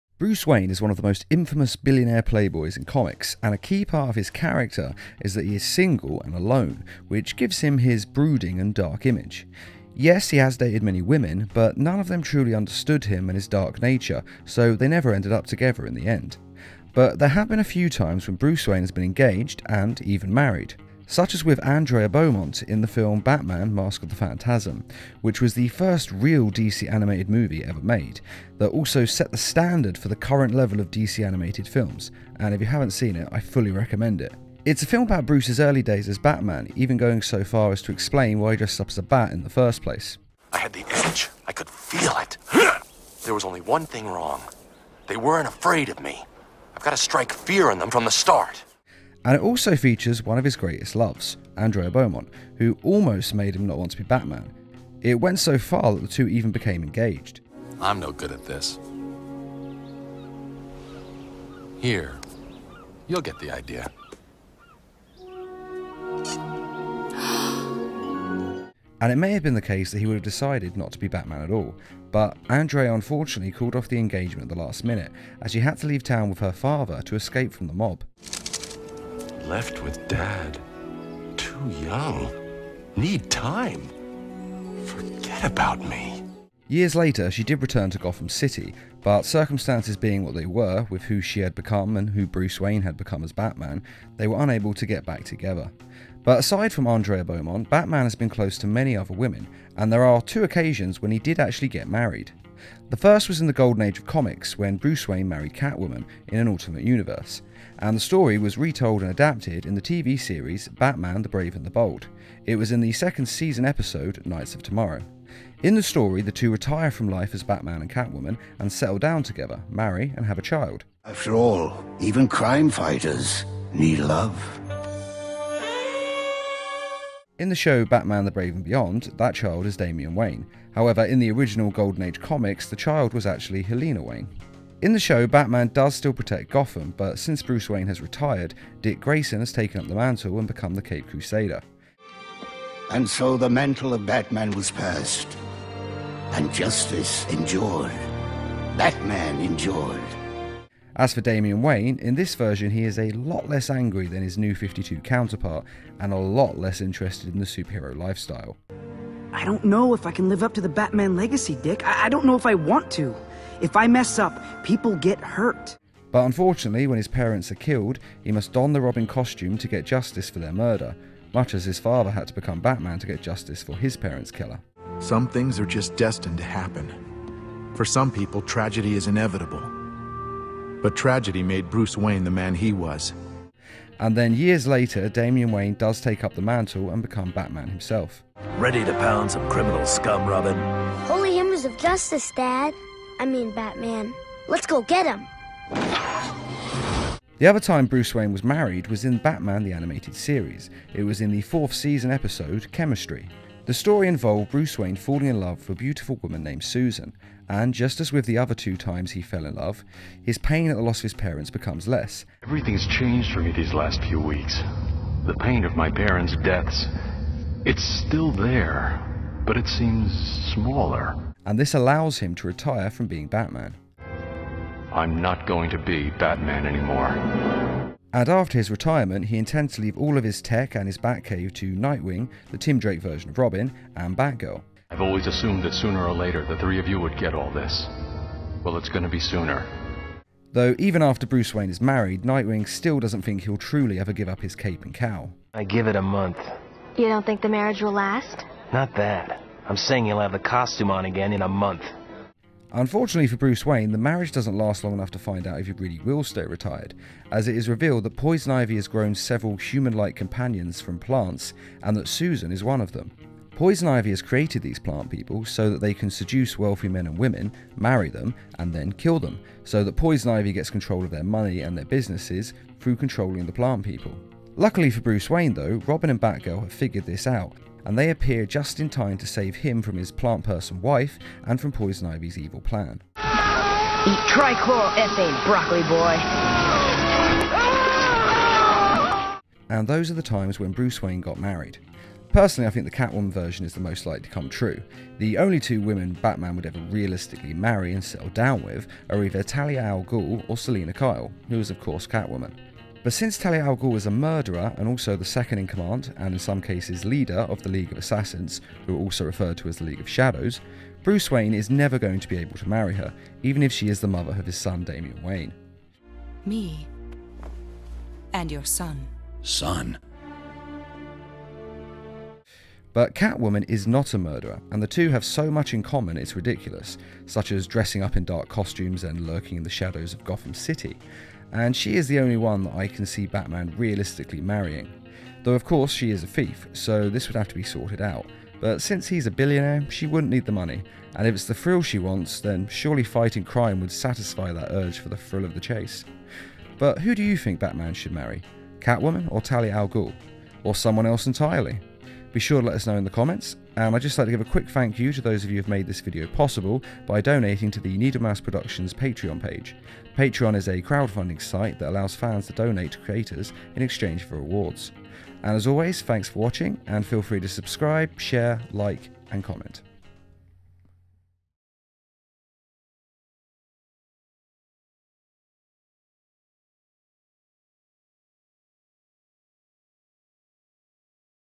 Has Bruce Wayne Ever Been Married?
Bruce Wayne is famous for being a bachelor but even Batman is vulnerable to the fairer sex and his marriage was inevitable.